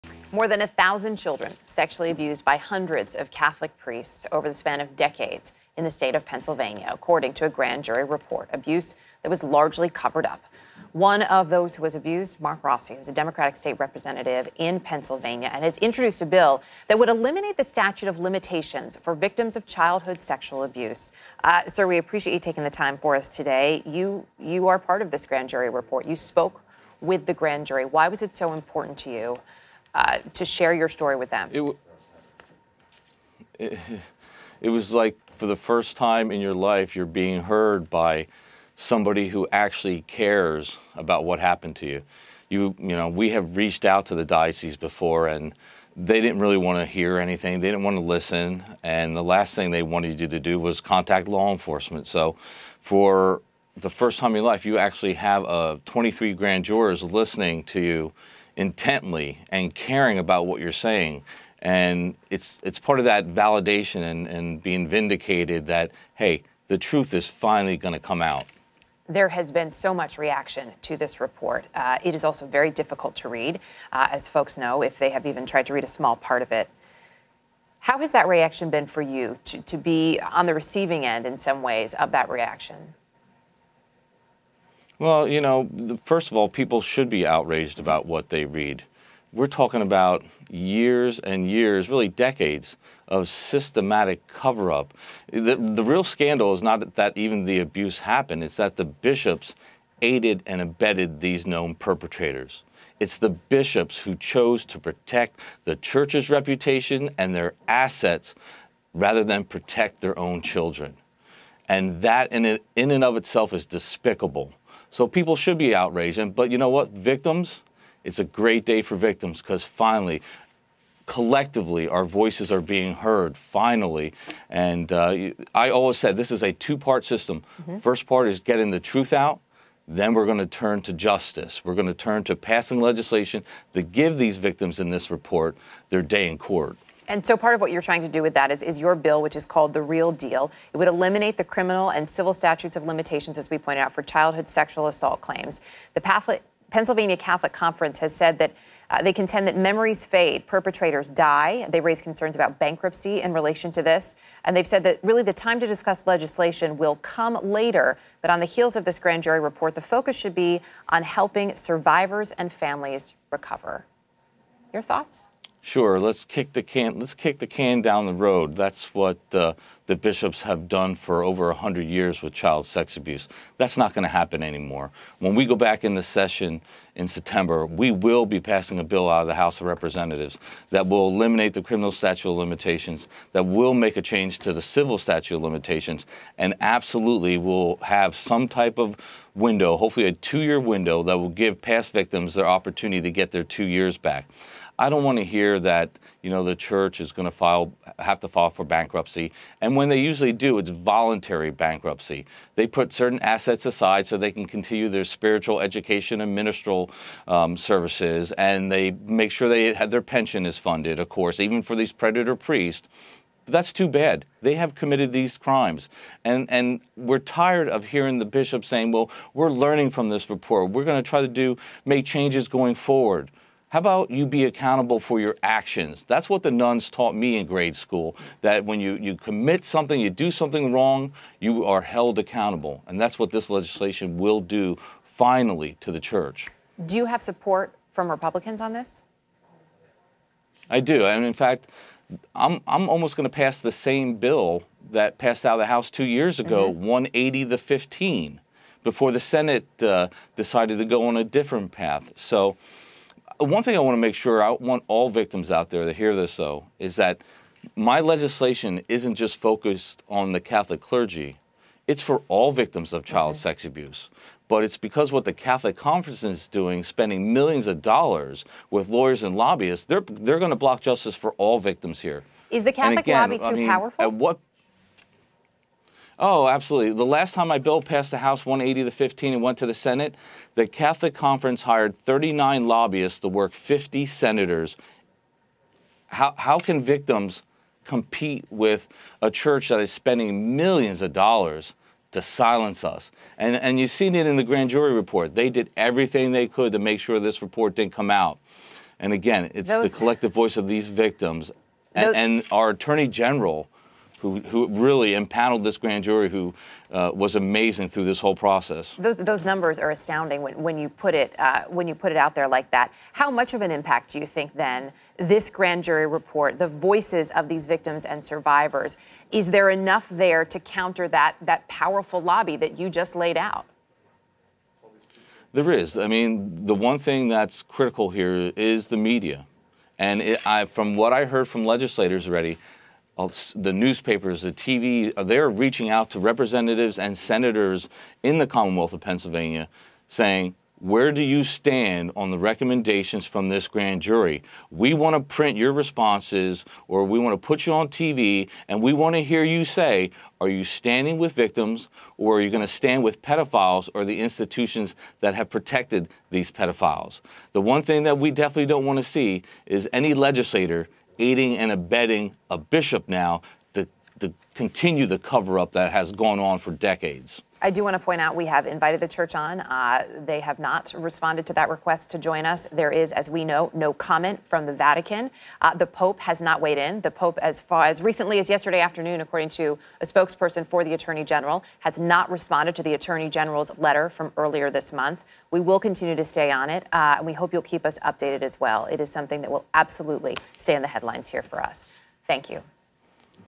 Priest sex abuse survivor: We're talking about decades of systematic cover-up
A grand jury report says that internal documents from six Catholic dioceses in Pennsylvania show that more than 300 "predator priests" have been credibly accused of sexually abusing more than 1,000 child victims.